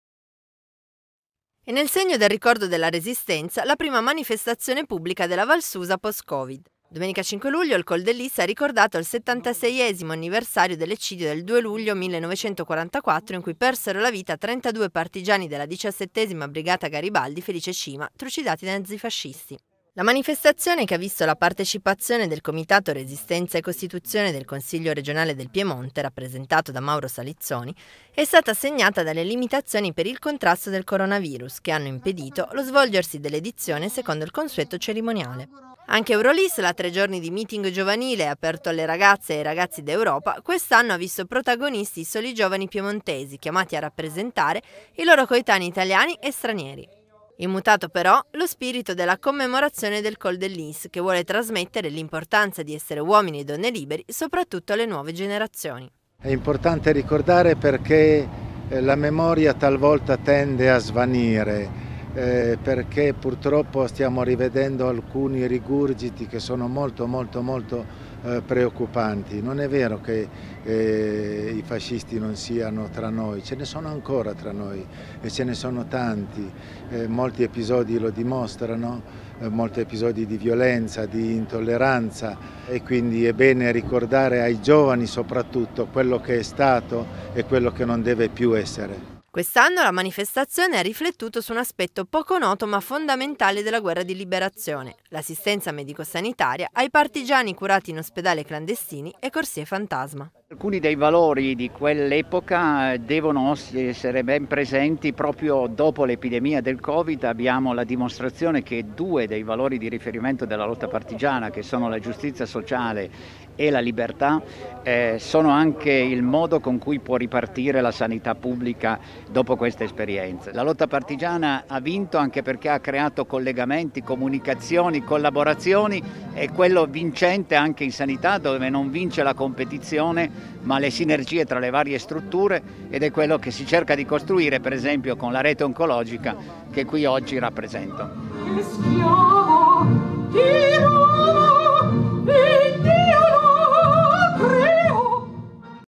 Il Col del Lys ricorda i suoi partigiani
Domenica 5 luglio il Col del Lys ha ricordato il 76° anniversario dell’eccidio del 2 luglio 1944 in cui persero la vita 32 partigiani della 17° Brigata Garibaldi “Felice Cima” trucidati dai nazifascisti. Un’edizione segnata dalle limitazioni per il contrasto del coronavirus, che hanno impedito lo svolgersi della manifestazione secondo il consueto cerimoniale.